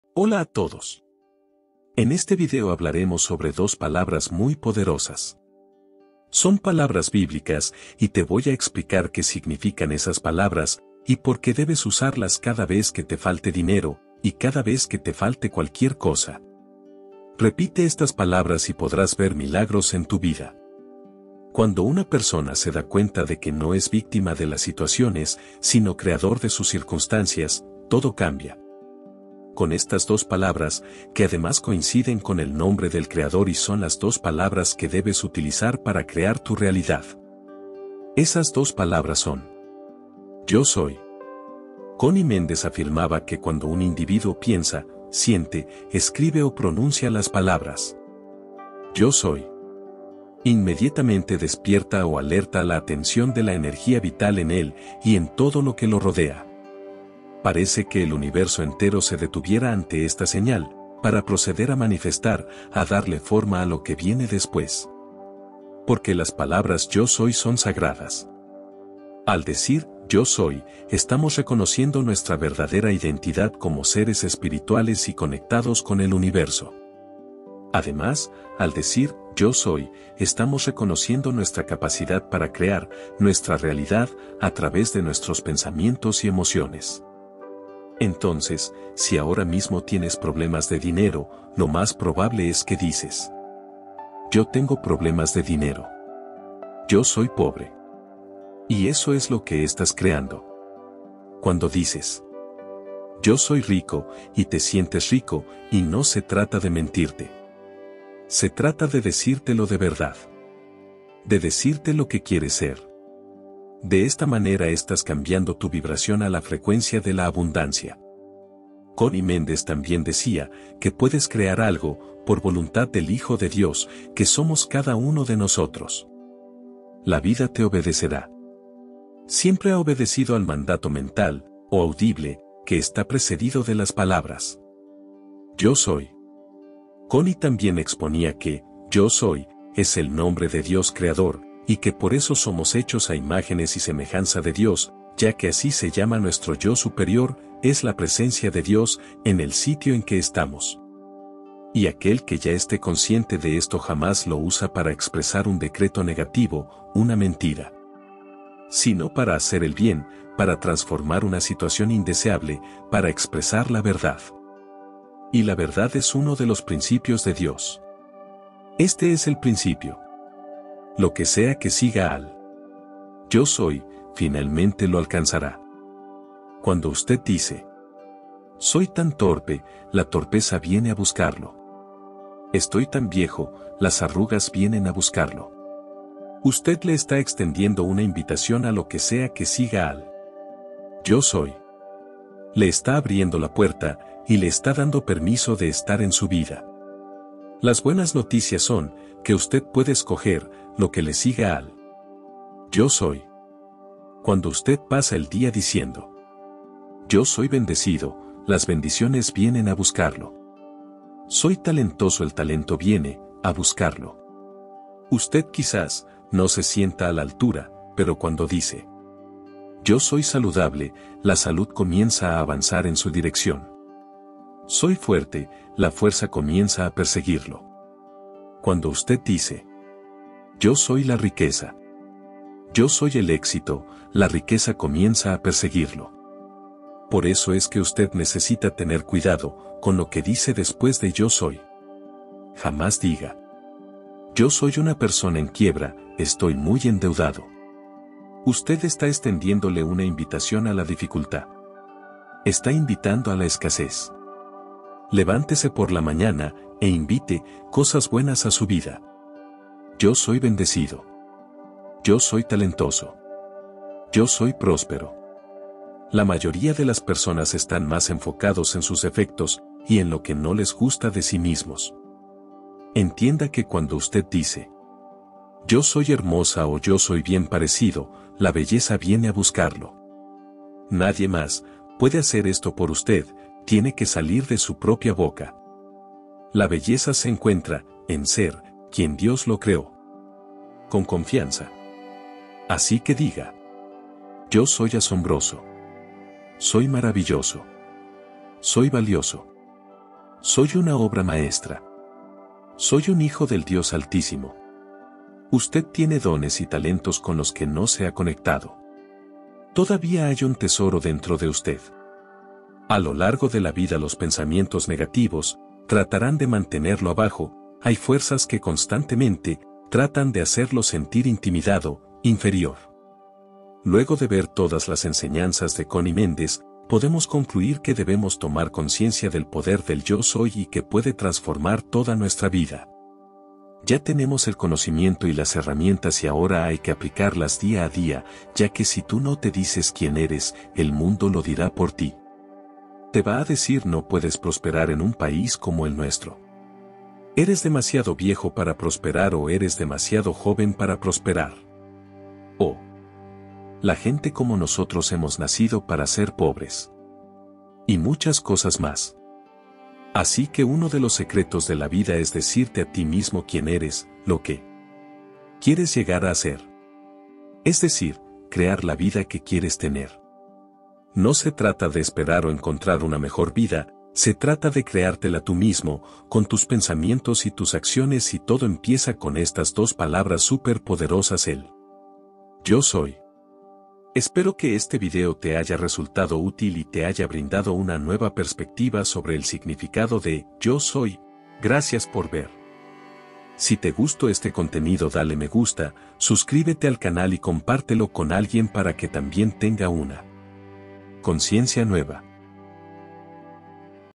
Solo Repite estas 2 PALABRAS.| (CONNY MENDEZ )
En este video hablaremos sobre dos palabras muy poderosas. Son palabras bíblicas, y te voy a explicar que significan esas palabras, y porque debes usarlas cada vez que te falte dinero, y cada vez que te falte cualquier cosa. repite estas palabras y podrás ver milagros en tu vida.
Con estas dos palabras, que además coinciden con el nombre del creador y son las dos palabras que debes utilizar para crear tu realidad. Esas dos palabras son.  YO SOY.
Conny Méndez afirmaba que cuando un individuo piensa, siente, escribe o pronuncia las palabras. yo soy. inmediatamente despierta o alerta la atención de la energía vital en él, y en todo lo que lo rodea. parece que el universo entero se detuviera ante esta señal, para proceder a manifestar.
Conny Méndez.
Yo Soy .
Abundancia.